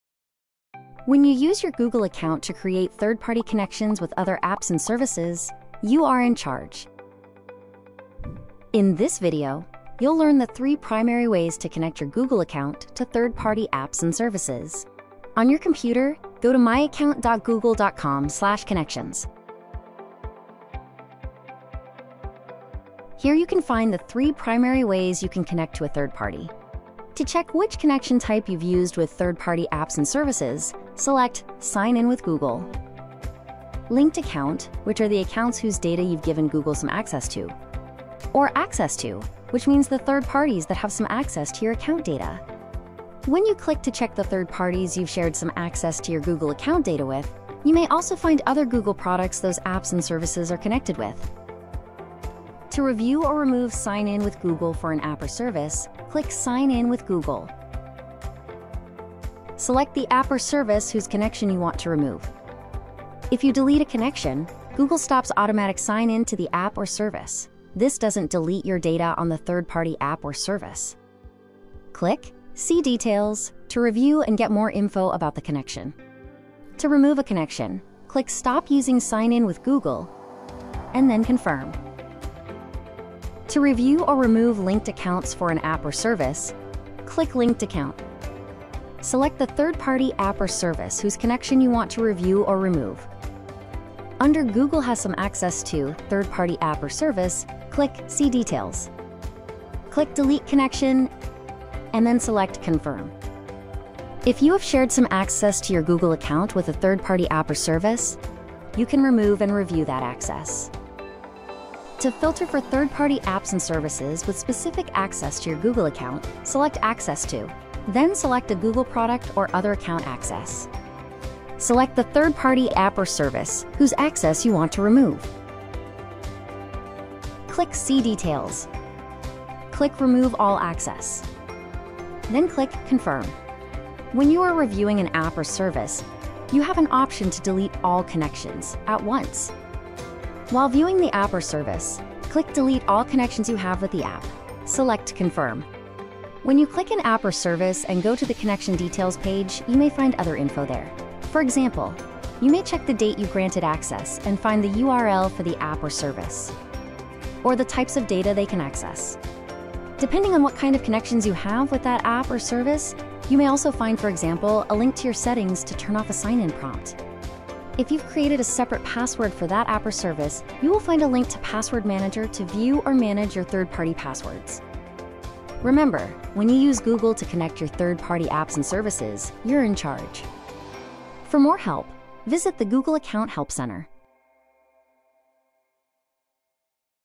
Third-party connections: You're in charge
Want to manage your third-party connections? Learn how to review or remove third-party apps and services connected to your Google Account, including:
Apps and services you can sign in to using your Google Account.
Linked accounts to which you've given Google some data access.
Third parties that have some access to your account data.

00:16 - Check which connection type you’ve used with third party apps and services.
00:53 - Review or remove automatic "Sign in with Google" for third-party apps and services.
01:23 - Review or remove linked accounts for third party apps or services.
1:52 - Filter and remove third-party apps and services with specific access to your Google Account
02:14 - Option to delete all connections of third party apps or services at once.
02:27 - Check for the date access was granted, the app or service URL, and the data access types of third-party apps and services.
2:49 - Find a link to Password Manager to view or manage your third party apps or services saved passwords.